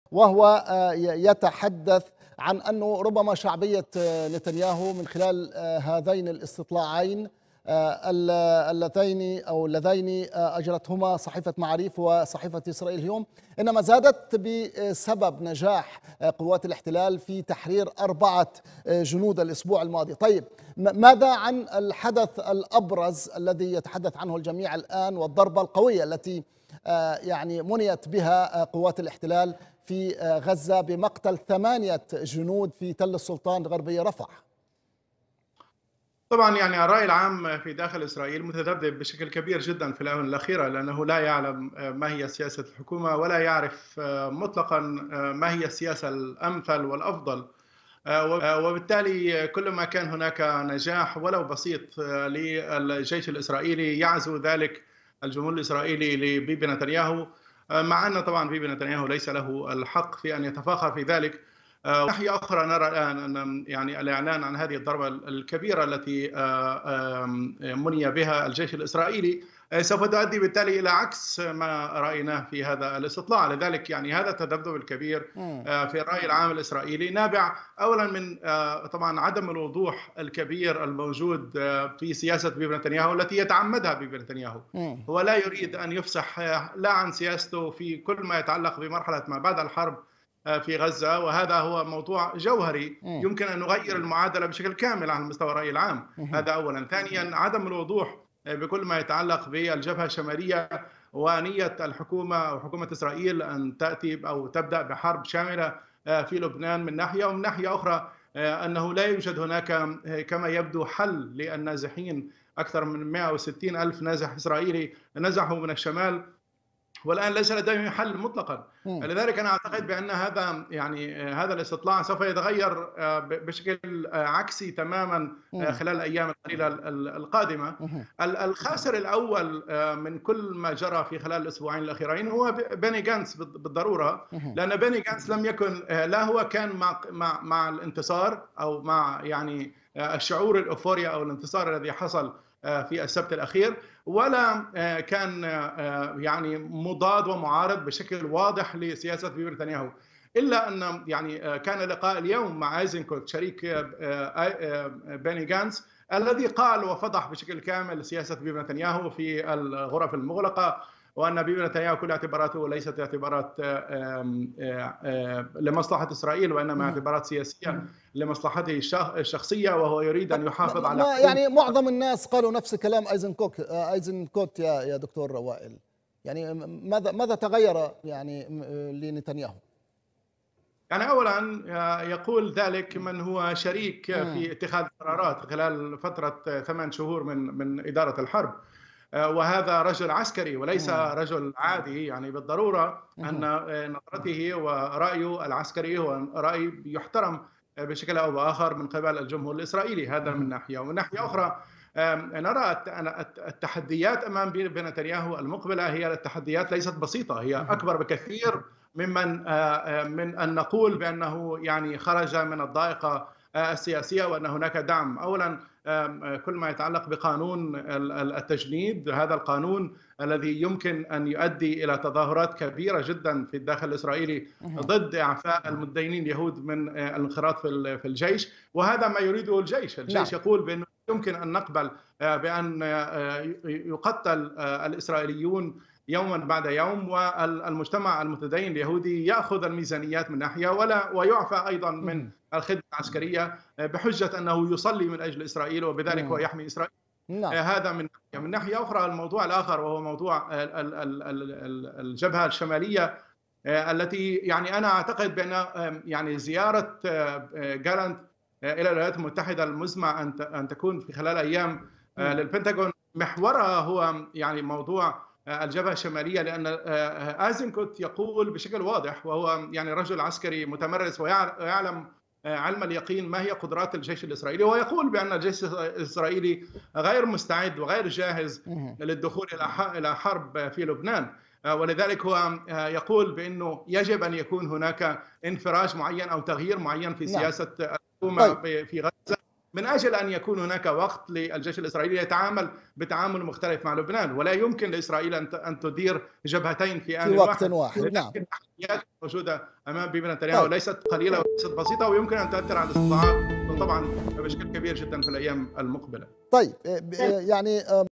خبير: الشارع الإسرائيلي يعيش حالة من التذبذب لهذه الأسباب
مستمرون في تغطيتنا من قناة الغد للعدوان الإسرائيلي على قطاع غزة.. وللمزيد من التفاصيل ينضم الينا الدكتور وائل كريم، الأكاديمي والباحث السياسي، مع الإعلامي عمرو عبد الحميد عبر